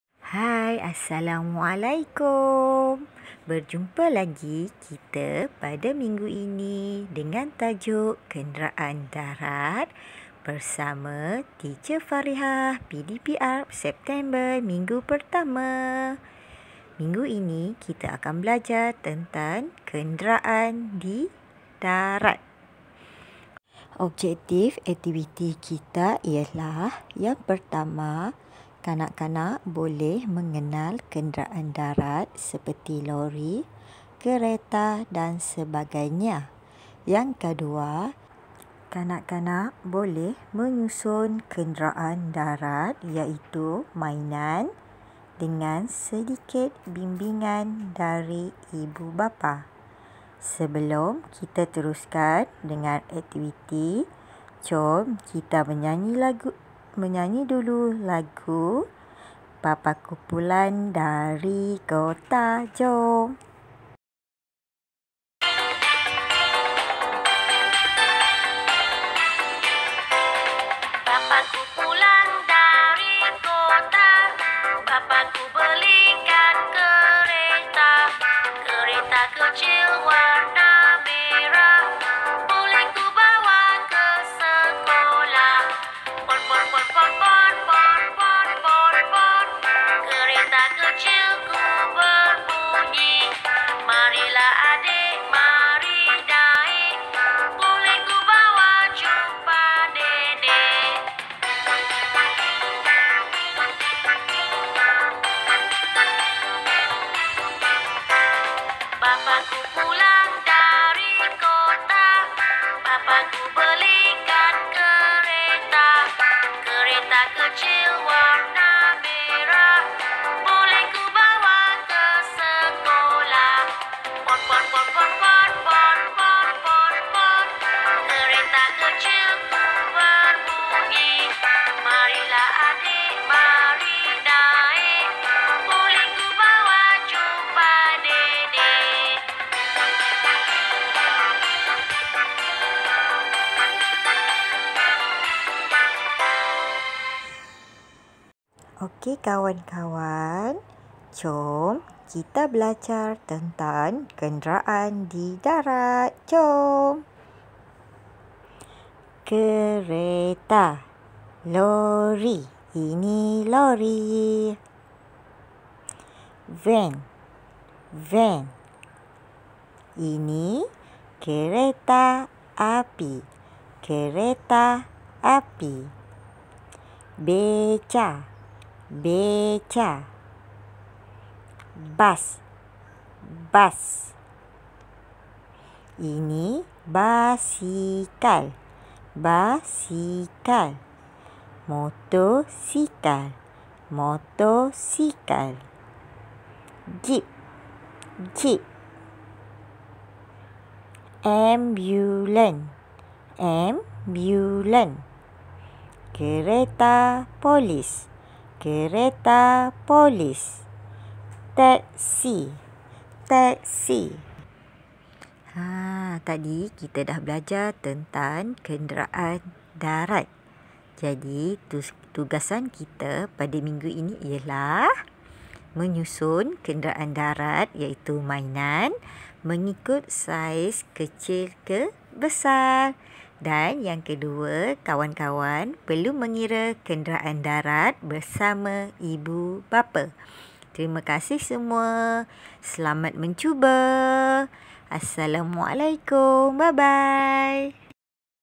Teacher Farihah - Kenderaan Darat
Hi kawan-kawan. Untuk tema bulan ini iaitu kenderaan, Teacher Farihah ingin mengajak kawan-kawan untuk mengenali beberapa kenderaan darat. Mari kita belajar bersama-sama.

Video Pembelajaran : 2 tahun
Pendidik : Teacher Farihah
Perkembangan : Deria & Pemahaman Dunia Persekitaran